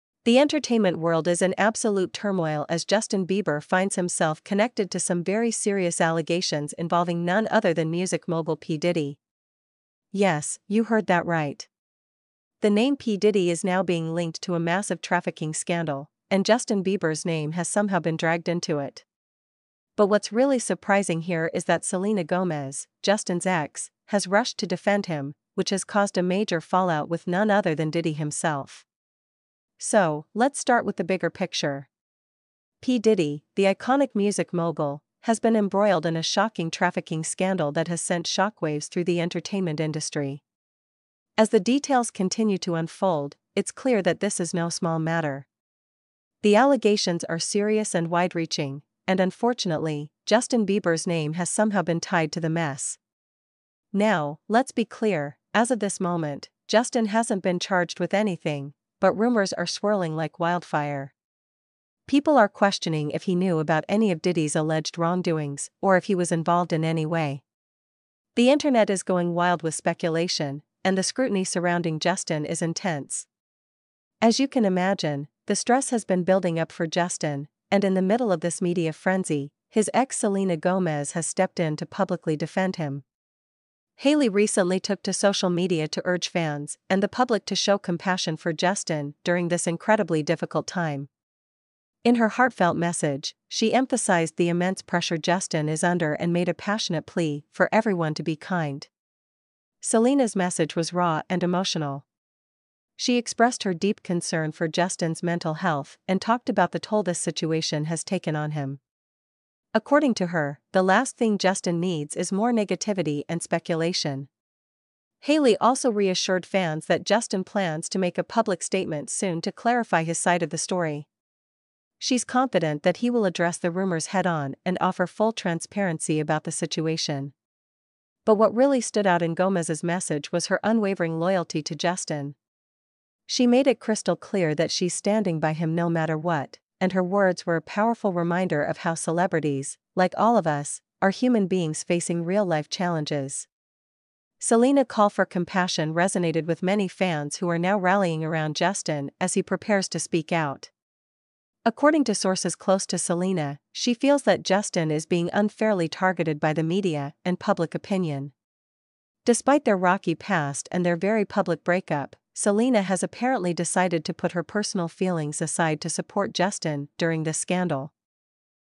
Selena Gomez RUSH To DEFEND Ex Justin Bieber FACES Major FALLOUT With P Diddy SCANDAL
The entertainment world is in absolute turmoil as Justin Bieber finds himself connected to some very serious allegations involving none other than music mogul P. Diddy. Yes, you heard that right. The name P. Diddy is now being linked to a massive trafficking scandal, and Justin Bieber’s name has somehow been dragged into it. But what’s really surprising here is that Selena Gomez, Justin’s ex, has rushed to defend him, which has caused a major fallout with none other than Diddy himself. So, let’s start with the bigger picture. P. Diddy, the iconic music mogul, has been embroiled in a shocking trafficking scandal that has sent shockwaves through the entertainment industry. As the details continue to unfold, it’s clear that this is no small matter. The allegations are serious and wide-reaching, and unfortunately, Justin Bieber’s name has somehow been tied to the mess. Now, let’s be clear: as of this moment, Justin hasn’t been charged with anything, but rumors are swirling like wildfire. People are questioning if he knew about any of Diddy’s alleged wrongdoings or if he was involved in any way. The internet is going wild with speculation, and the scrutiny surrounding Justin is *intense*. As you can imagine, the stress has been building up for Justin, and in the middle of this media frenzy, his ex Selena Gomez has stepped in to publicly defend him. Hailey recently took to social media to urge fans and the public to show compassion for Justin during this incredibly difficult time. In her heartfelt message, she emphasized the immense pressure Justin is under and made a passionate plea for everyone to be kind. Selena’s message was raw and emotional.